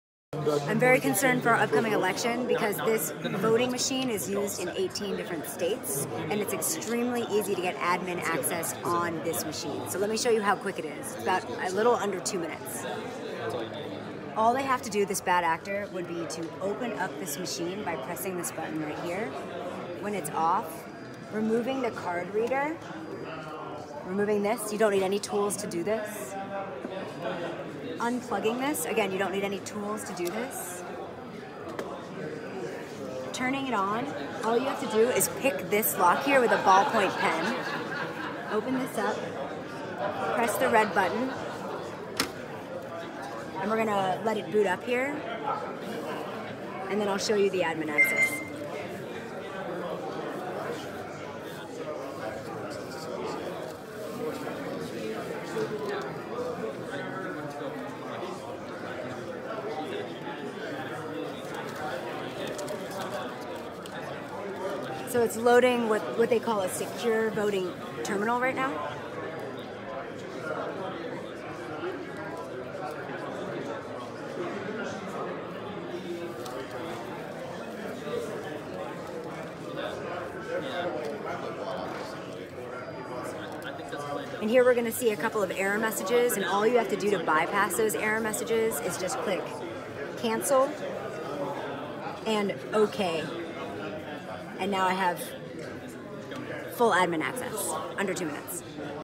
Rachel Tobac at the Defcon conference showed how easy it is to gain admin access to a voting machine
Rachel Tobac at the Defcon conference (2018) showed how easy it is to physically gain admin access on a voting machine that is used in 18 states.

Requires no tools and takes under 2 minutes.